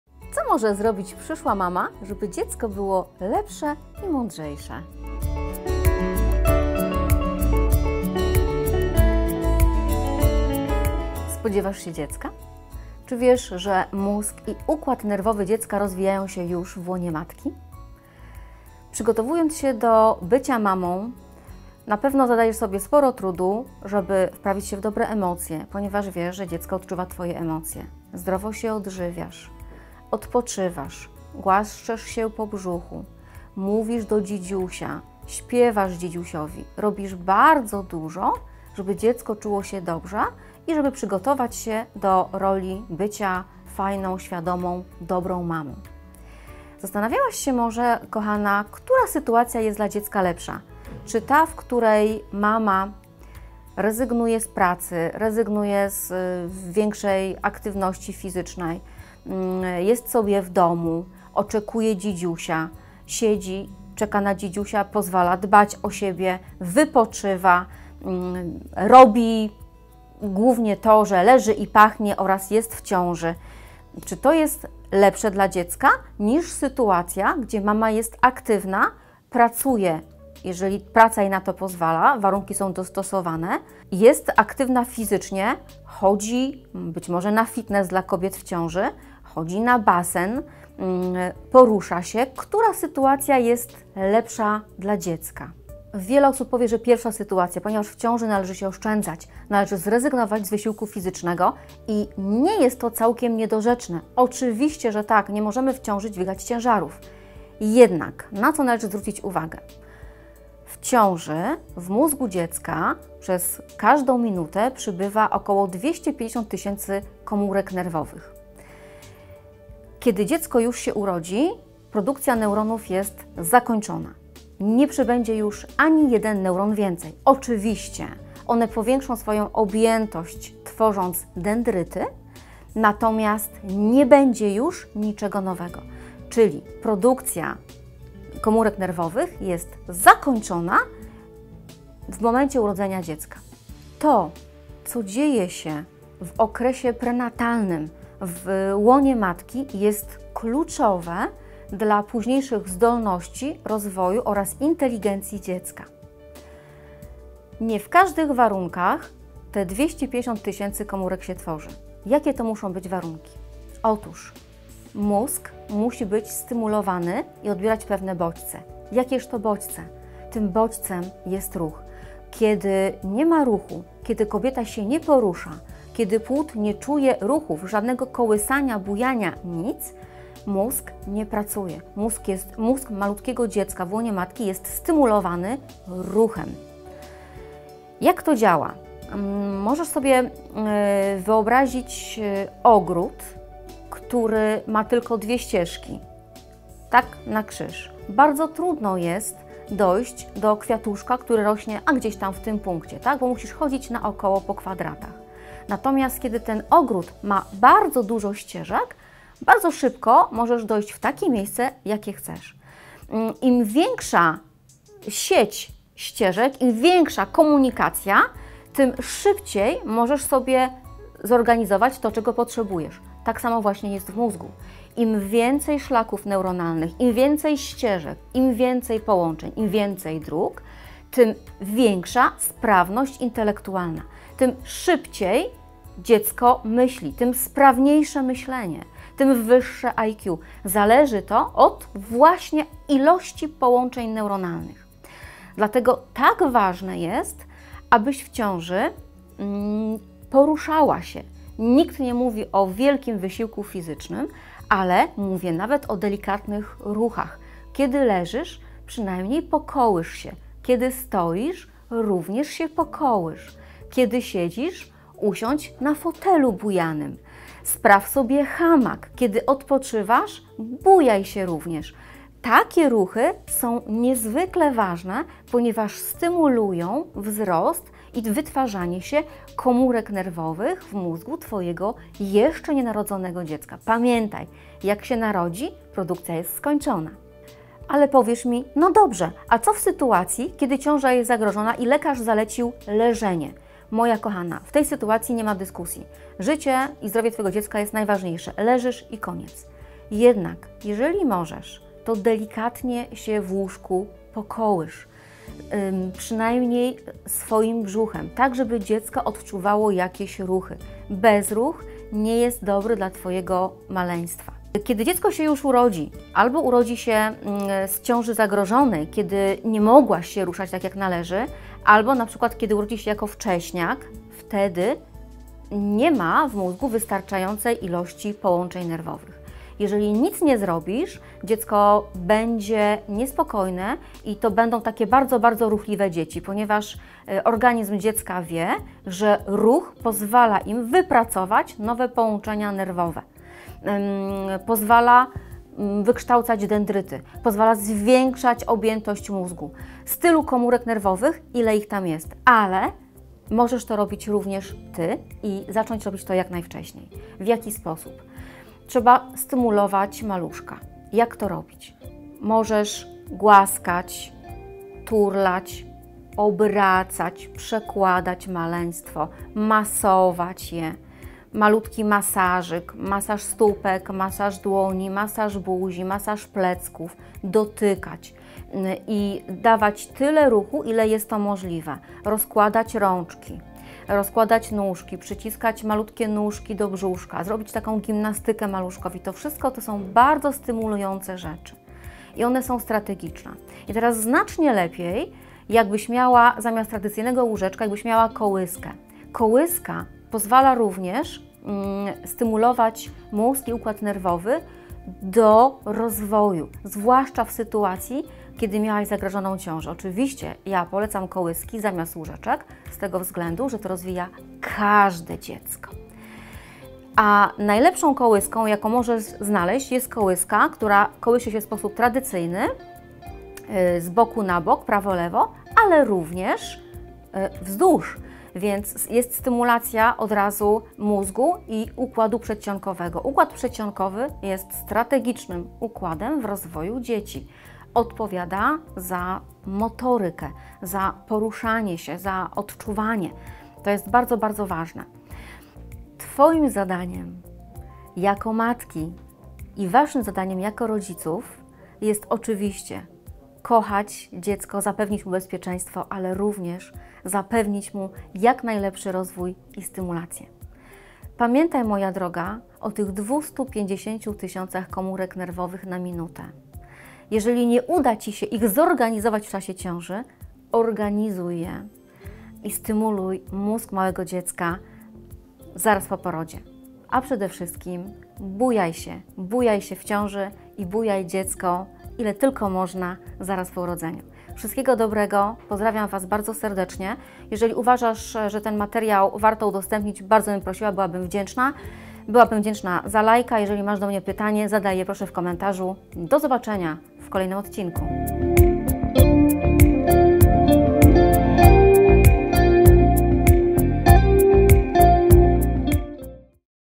Co może zrobić przyszła mama, aby jak najlepiej rozwijać swojego maluszka już w brzuchu!
Spodziewasz się dziecka? Twój maluszek niebawem przybędzie na świat? W jaki sposób najlepiej wpłynąć na jego rozwój już wtedy, gdy jest w twoim brzuszku?

Obejrzyj film i daj mi znać, czy wiedziałaś jakie TO ma znaczenie! 👆🏻😎